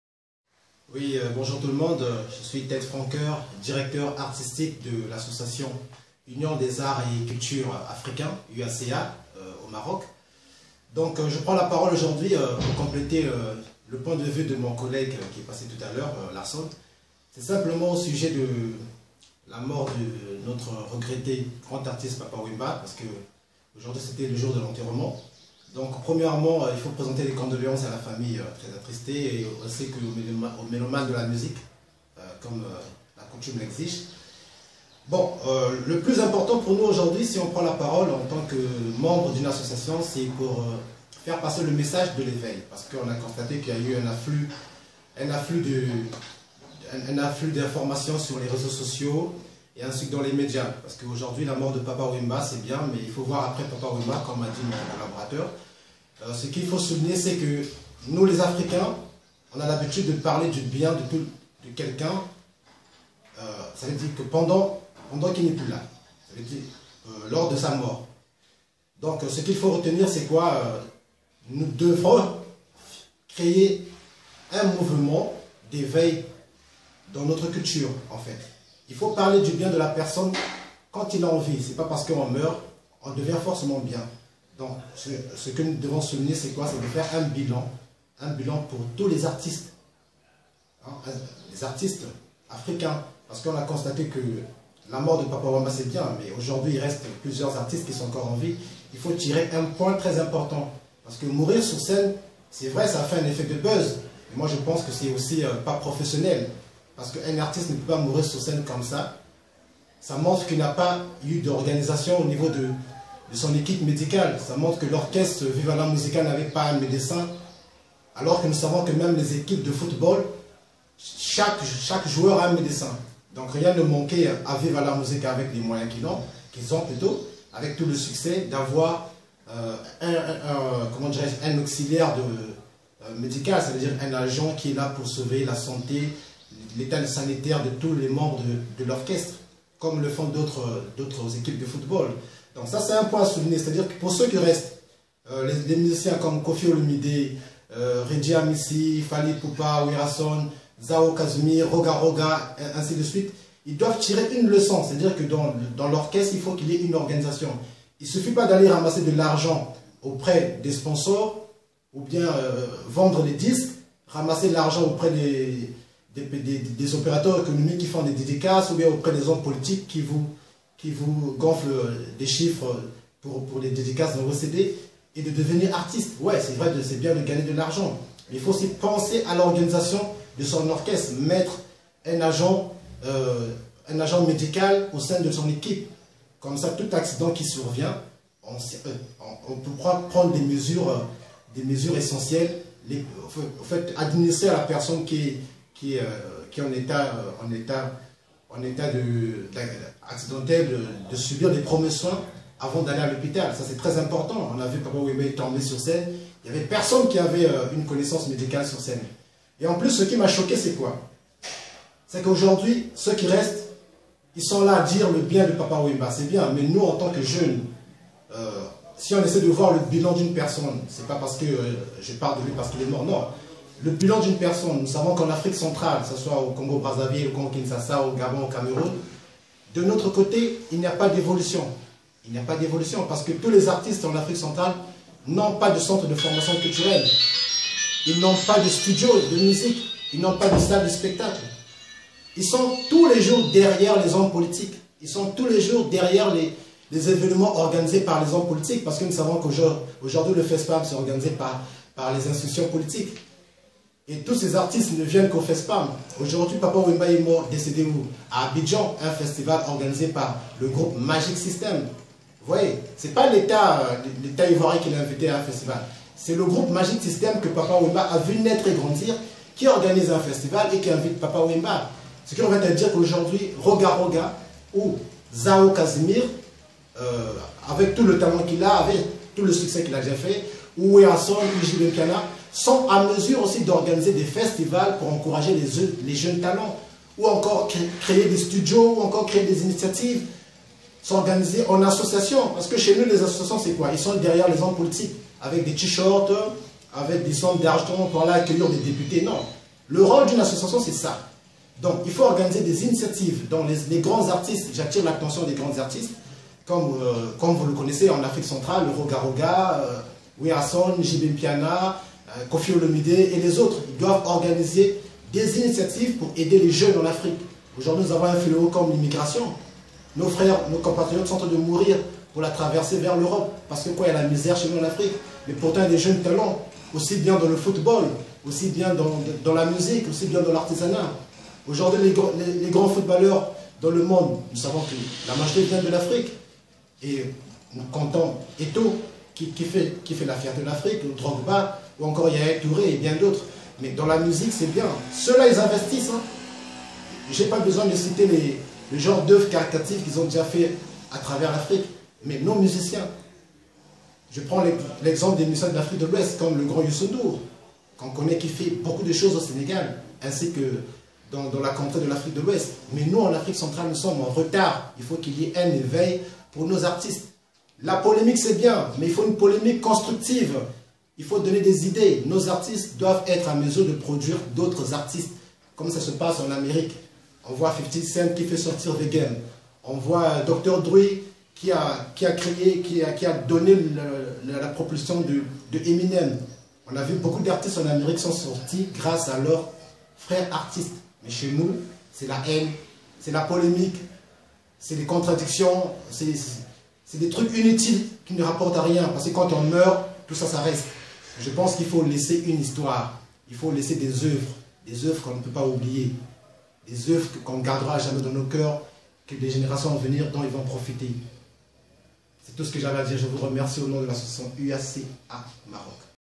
La mort de Papa Wemba - Point de presse UACA partie 2
Réflexion sur le bilan après la mort du grand baobab Papa Wemba